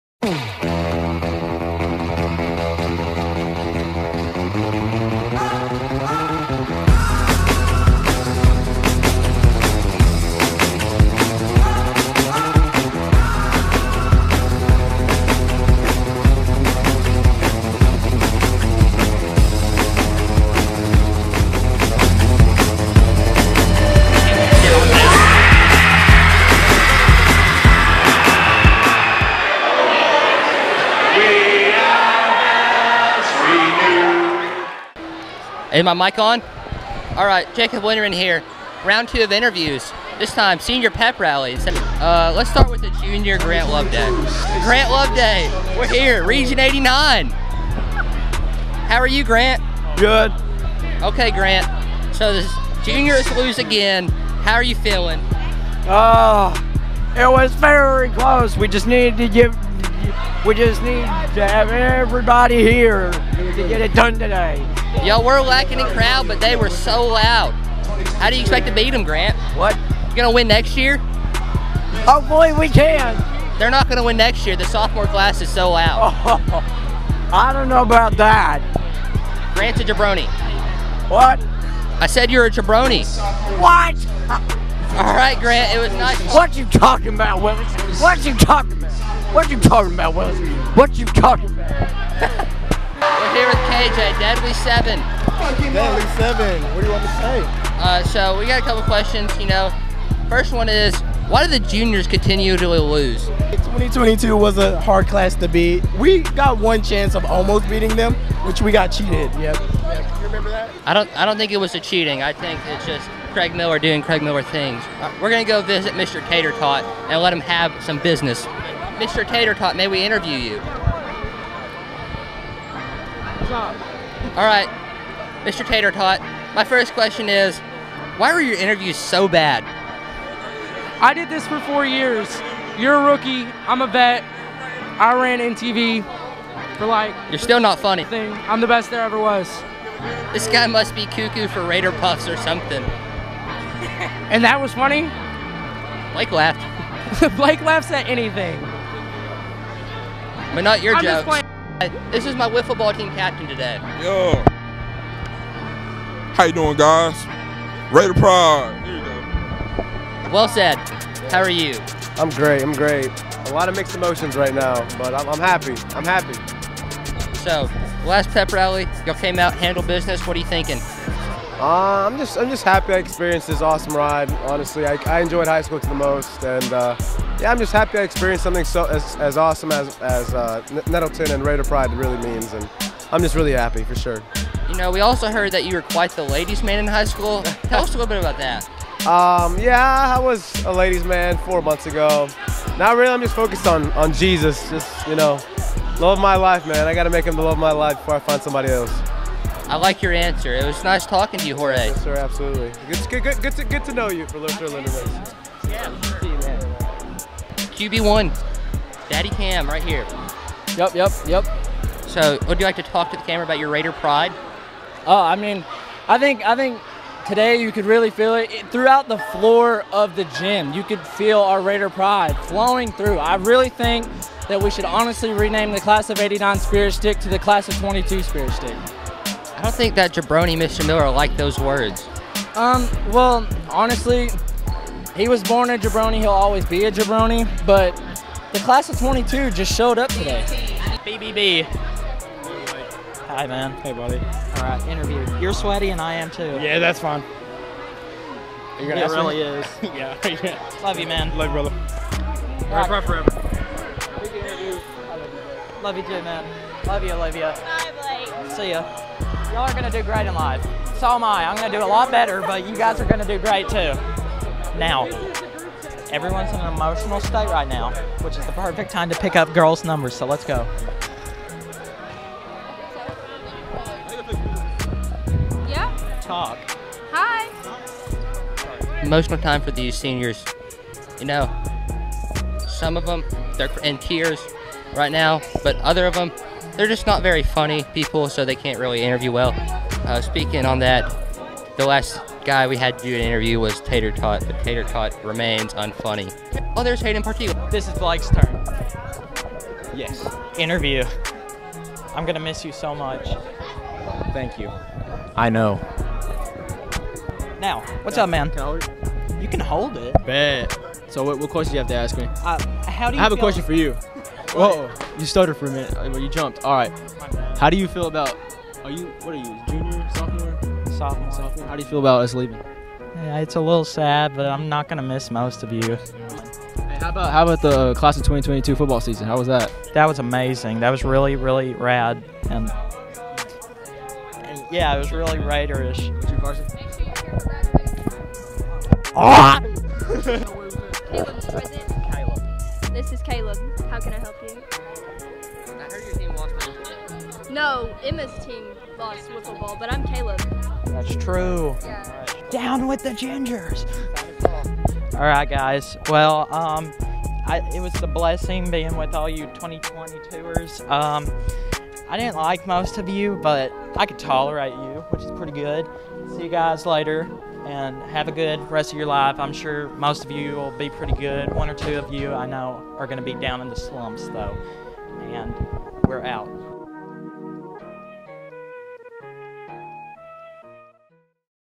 Region 89 Senior Pep Rally '22
Blake and Jacob (in his Region 89 debut) interview students at the Class of '22's final pep rally.